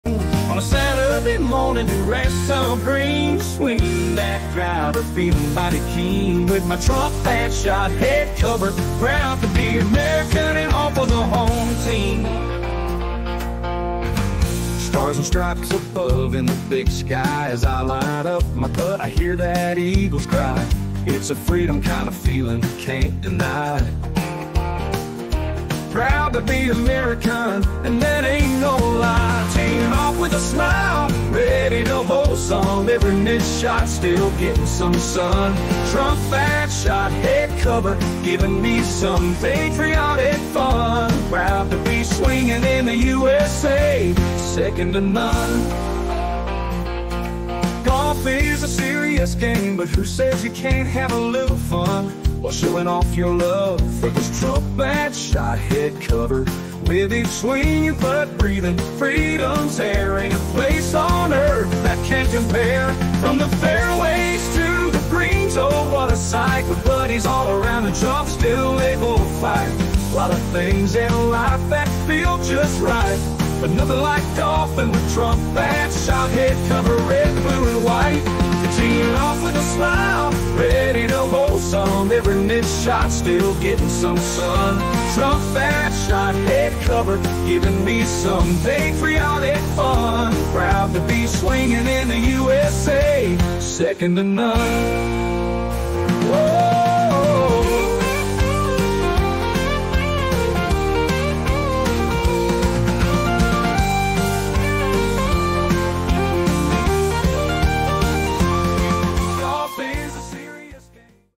Trump ‘BAD SHOT’ Golf Headcovers: The Ultimate Patriotic Golf Accessory You Need!
🚨🎵 NEW HIT ALERT! 🎵🚨

Introducing the most talked-about golf headcovers of the year: the Trump "BAD SHOT" Headcovers! 🇺🇸🤣  And guess what? They’ve got their own country hit song now! 🎤🤠

From birdies to bogeys, add some laughs to your game and a little red, white, and blue to your bag! ⛳️🔥

Get ready to tee off with a grin – ‘cause golf is way better when you bring the fun! 😂 🙌.

Or go to Amazon.com and search Trump “BAD SHOT Headcovers”.